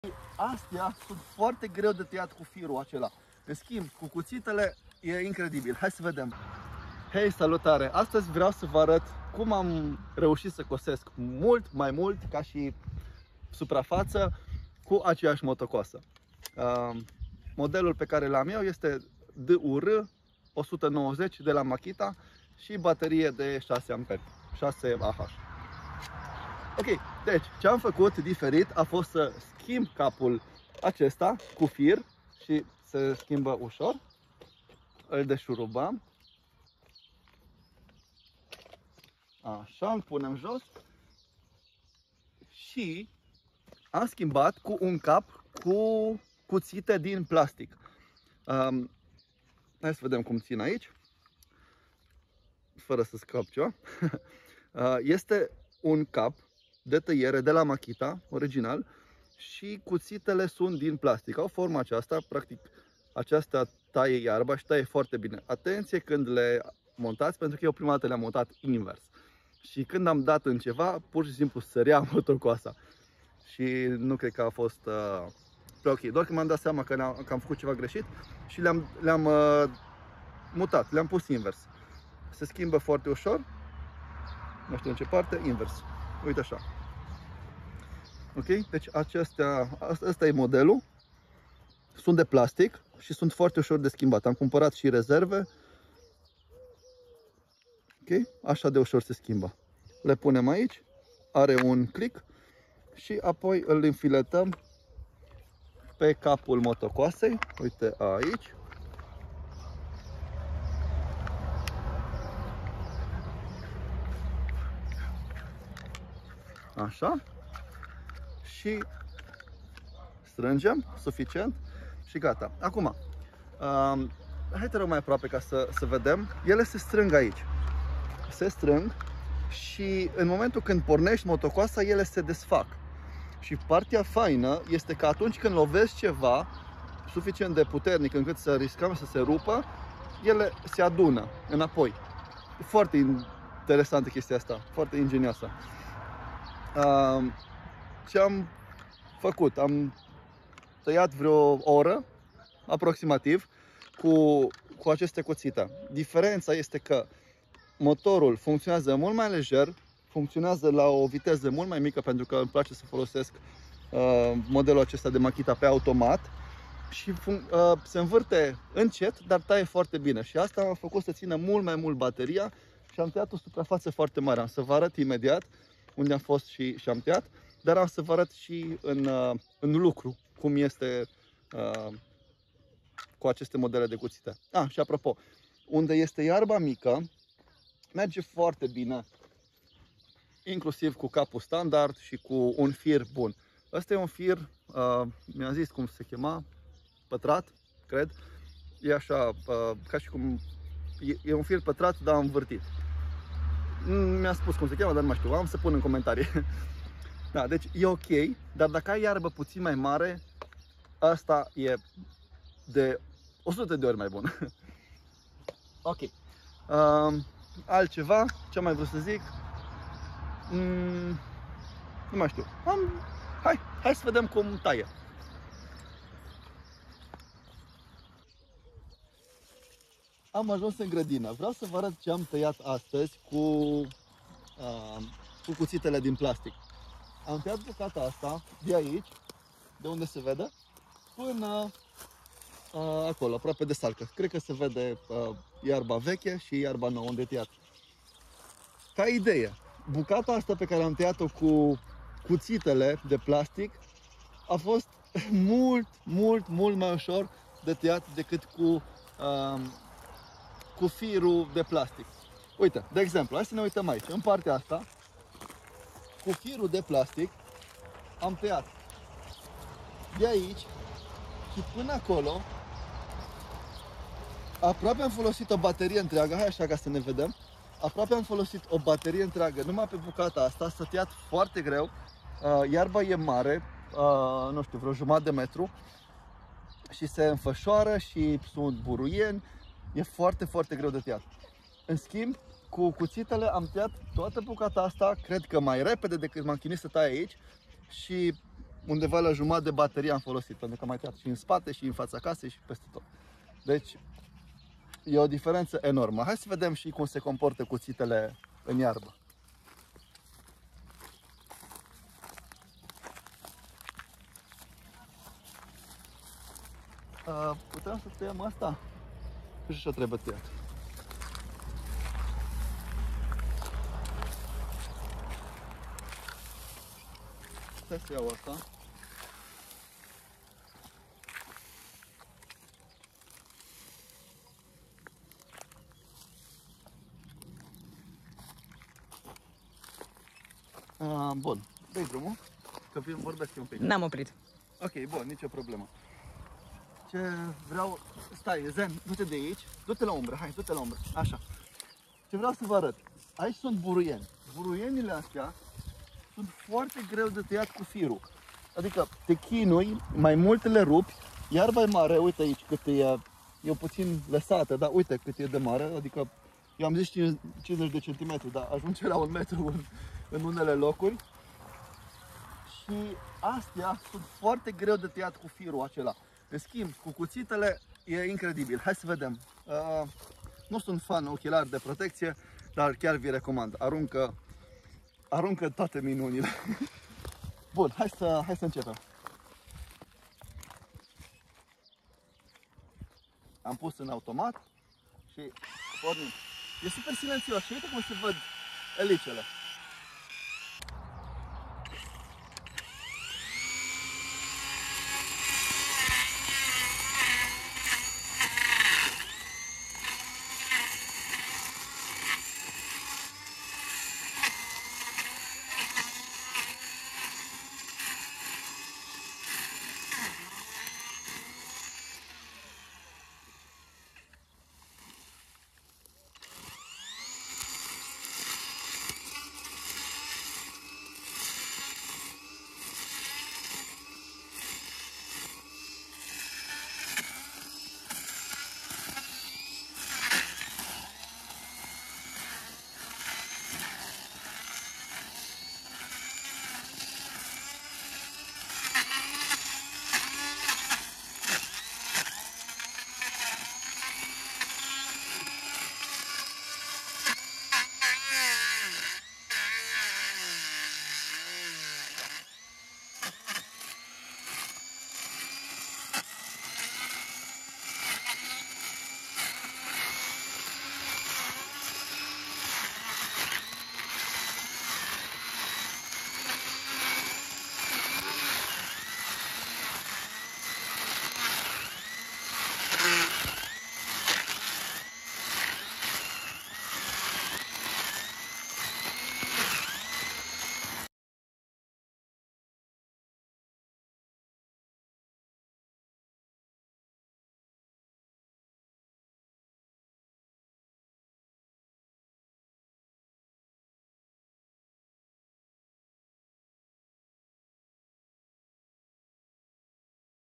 Makita trimmer electric cu acumulator baterie si cutite din plastic in loc de fir
Cum am reusit sa tai mai multa iarba cu acelasi trimmer Makita

Hey salut si bine ai venit in acest video unde iti arat cum am reusit sa tai mai multa iarba cu mai putin efort si consumand mai putina baterie.

La inceput am folosit firul primit la achizitia trimmerului, care sincer a fost bunicel. Apoi am cumparat un fir patrat, mai performant care a fost si el de calitate buna ... dar tot aveam probleme ca nu puteam sa tai suficienta iarba iar bateria tine aaproximativ 30-40 minute. Daca era iarba mare si cu buruiana mare, nu ajengeam sa tai 30 minute.

In schimb, acum am cumparat cutitele de la Makita, este modelul cu 3 cutite din plastic care se pot inlocui.

Imediat ce le-am pus pe trimmer si am iesit la taiat. WOW!

Ce diferenta.

Pare alta masinarie, si e pe bune.

Ca idee, recomand 100% sa iti iei aceste cutite pentru iarta mare si buruieni.

Daca vrei sa ma intrebi mai multe, scrie in comentarii :)

... sau pe facebook/ instagram : Angel Rodian

*nu sunt expert in gradinarit sau aparate de gradina, insa spun si impartasesc experienta mea cu scopul de a ajuta alte persoane care sunt mai la inceput decat mine si nu stiu ce sa aleaga.